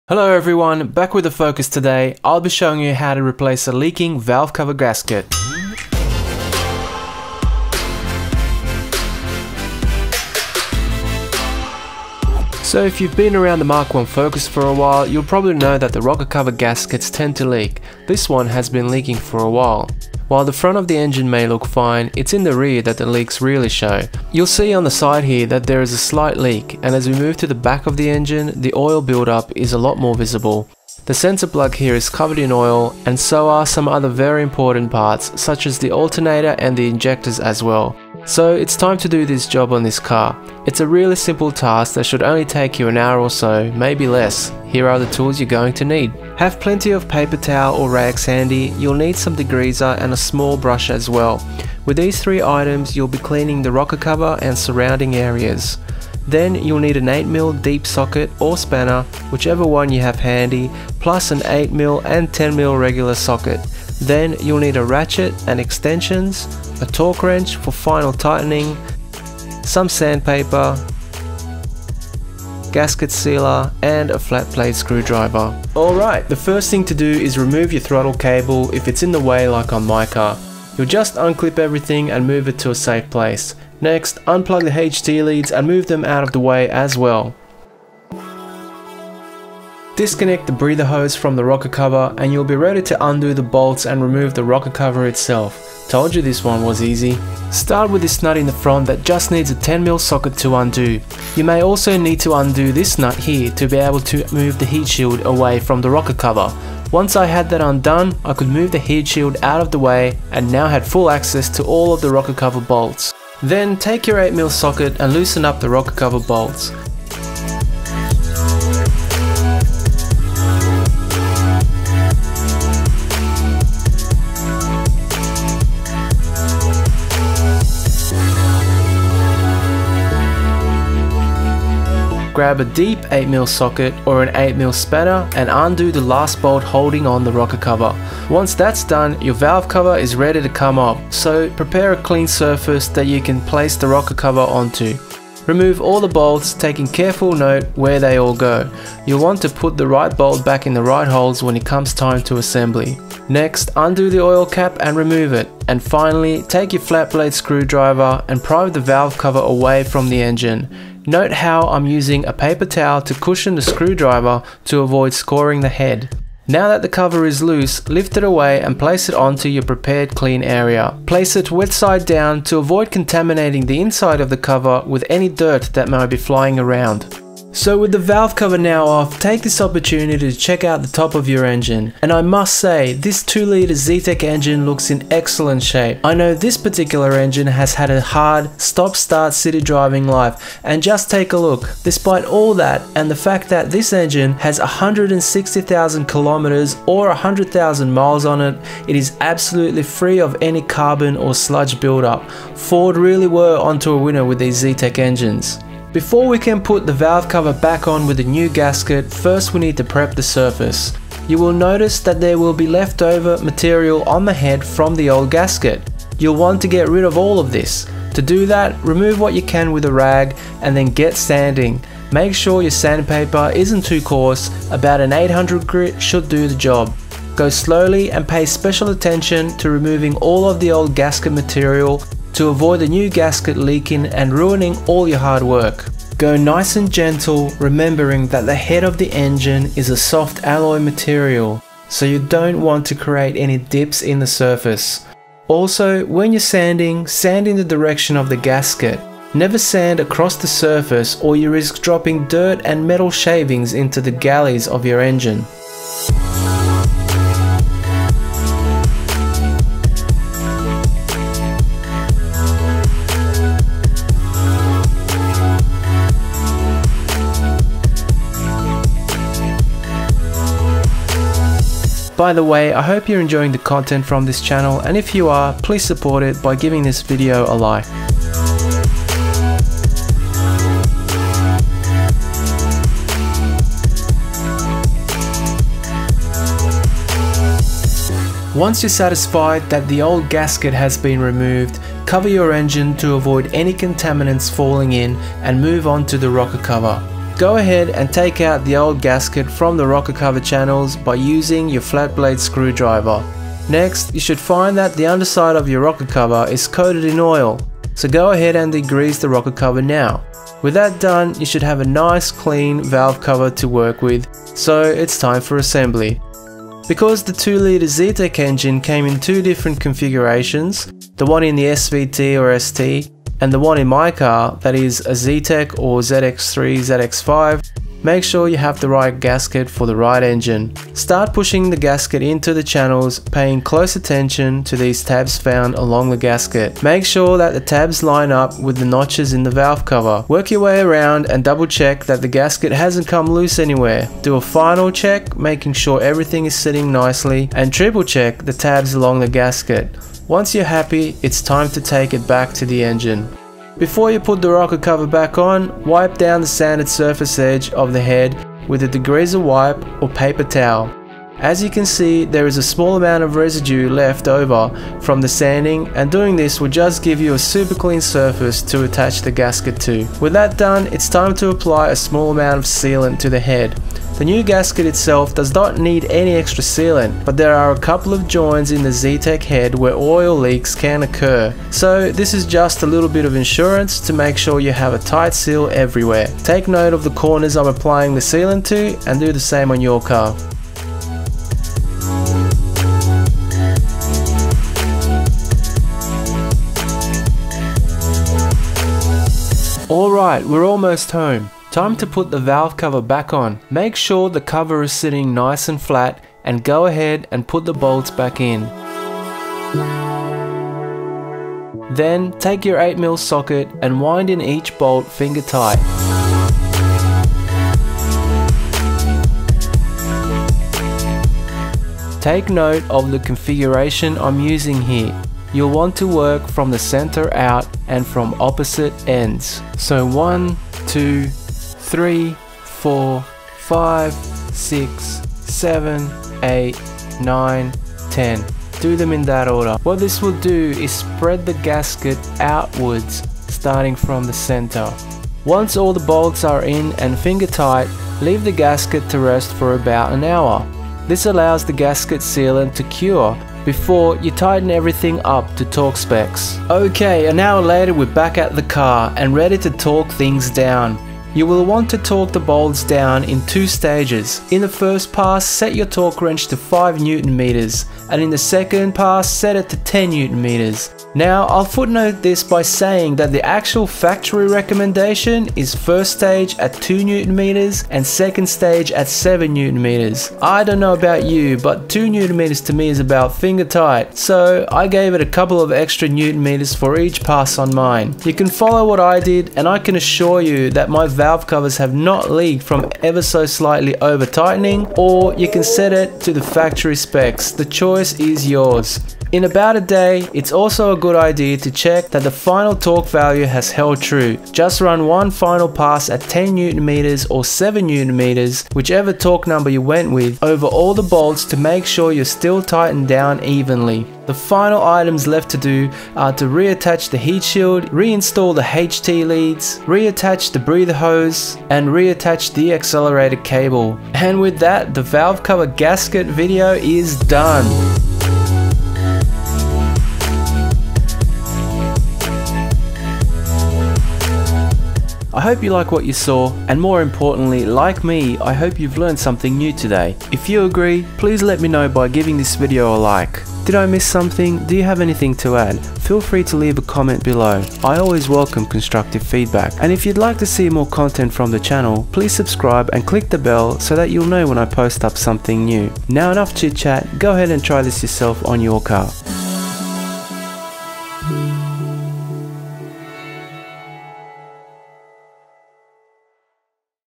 EASY: Valve Cover Gasket Replacement - Ford Focus Mk1 / LR
Valve Cover Gasket Leak or Rocker Cover Gasket Replacement Guide - Ford Focus Mk1 Zetec Engine.

I’ll show you every step required to change a leaking valve cover gasket, also known as a rocker cover gasket, on a Mk1 Focus Zetec Engine.

I list all the tools needed to change the gasket, as well as handy tips for tightening down the final bolts to correct torque specs. I also cover a small but important detail with gasket sealant that all Zetec engines should have done when putting in a new valve cover gasket.

This is a quick job that should take about 1 hour work total, and I detail every step here.

Links to recommended products:
Gasket Sealant